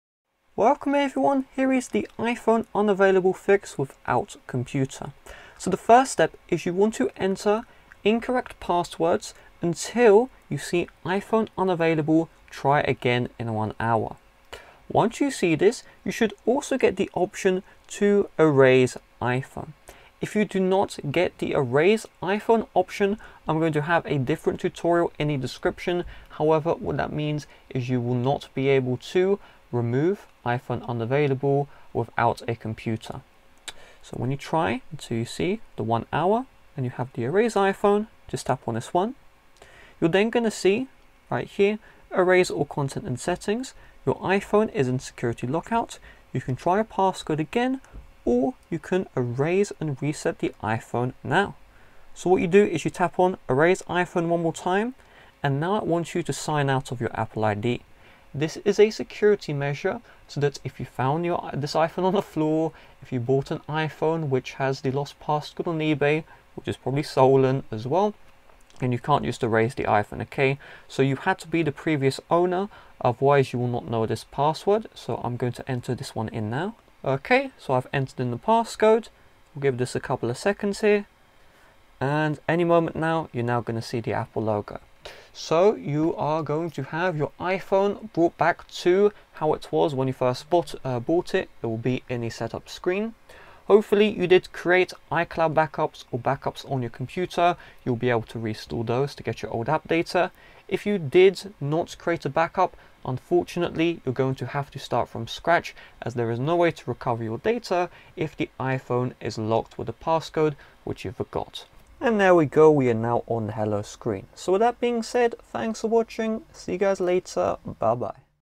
iPhone Unavailable FIX Without Computer
iPhone unavailable fix without computer | How to fix iPhone unavailable on iPhone! Do you own the iPhone and want to fix iPhone unavailable? If so, this video is for you! I will teach you how to fix iPhone unavailable on iPhone in this tutorial. By the end of this guide, you will be able to fix and solve the iPhone unavailable problem, with using a computer or PC!